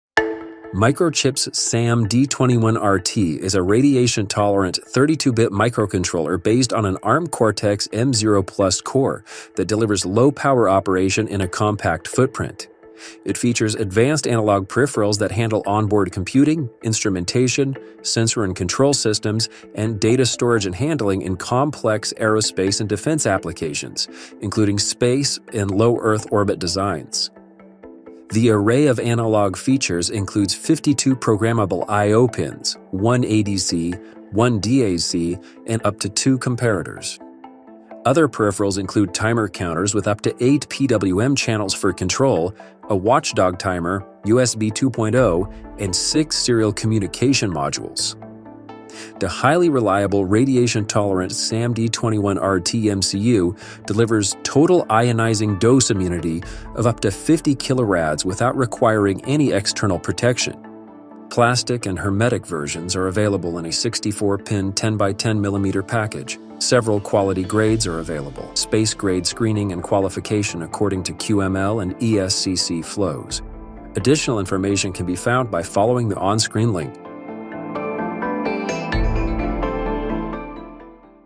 Strengthen Your Radiation-Tolerant Design with Our SAMD21RT MCU
As space technologies continue to become more advanced, our SAMD21RT MCU can help you meet stringent radiation and reliability standards to operate in the harsh environment. The SAMD21RT MCU features a robust Arm® Cortex®-M0+ based 32-bit microcontroller running at 48 MHz to deliver the highest processing power with low consumption and multiple low power modes.

This video will guide you through the exceptional features of this radiation-tolerant MCU. With 256 KB Flash memory and 32 KB SRAM, comprehensive peripherals and built-in hardware security, the SAMD21RT MCU supports a variety of real-world applications. Discover how our SAMD21RT MCU can enhance your designs and improve them with radiation performance to meet the needs of space missions.